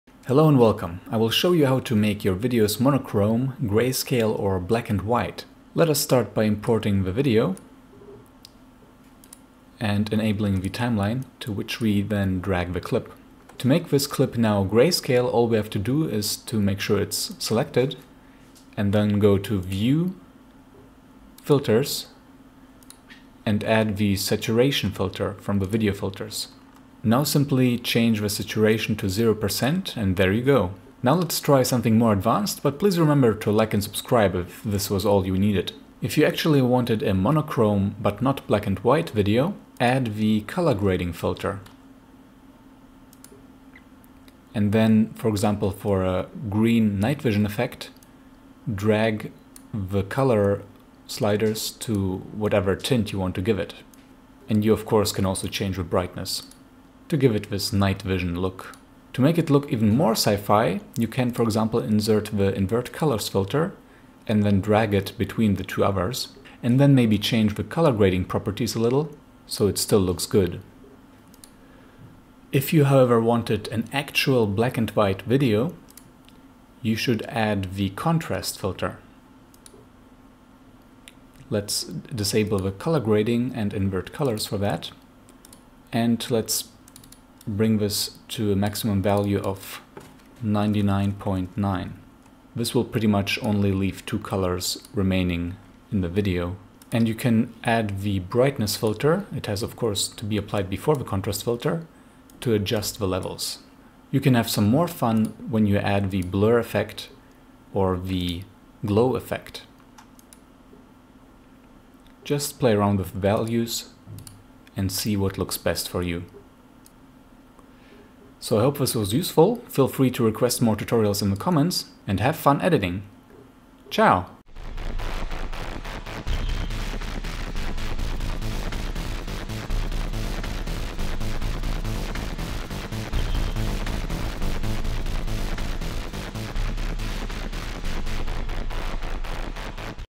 Shotcut Black & White, Grayscale, Monochrome Tutorial
To make a greysacle b&w effect or a cool night goggles vision filter on your video, all you need is the saturation filter, which you lower to 0% and then you can play with the values using the brightness and contrast effects, to even create a completely black and white video.

For the military green night vision, add the color grading filter and move its sliders to the green.

Add blur and/or glow to make it look even more special and with the invert filter you can make it seem even more otherworldly and sci-fi.